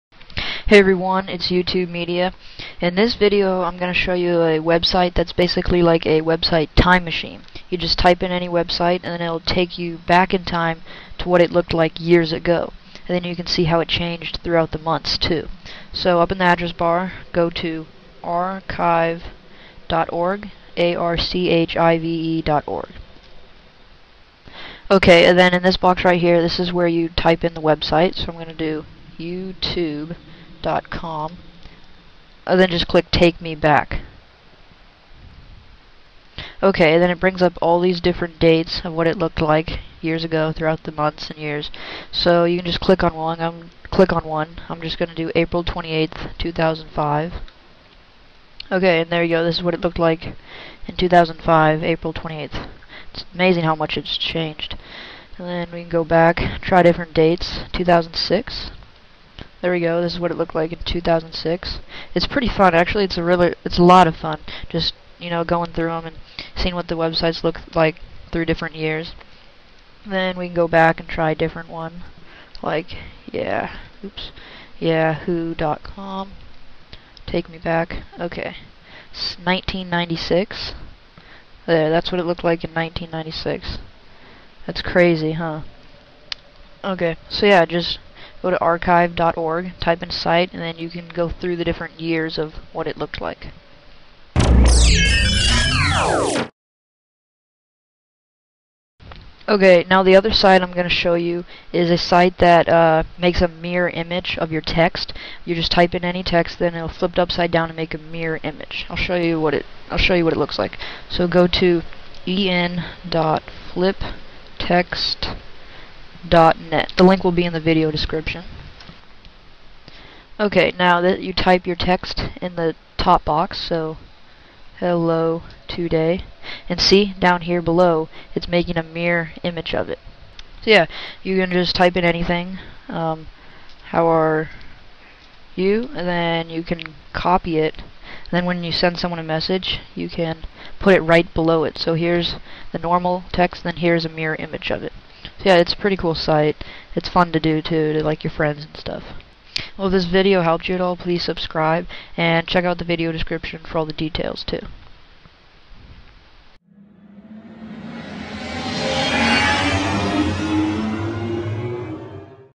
How to See Websites In The Past!
This video I show you how to go back in time with most websites, & see how crappy they looked years ago! You can choose different dates also. Then in the 2nd part, It's how to make a mirror reflection of any text you type, its' cool to send as a message!